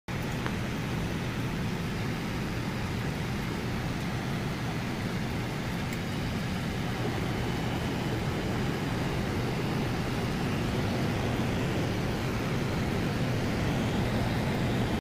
blue hippo tang dory eating
blue tang saltwater reef safe fish swimming and eating
hippo tang